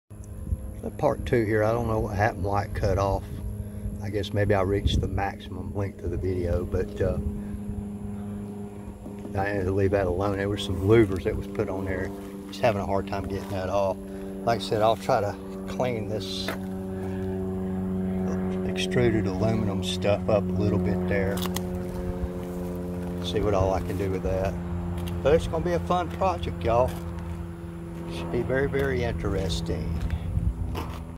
vwpart1a
Going to try and make this horrible, what looks like a backyard paint job look as good as possible-- helluva challenge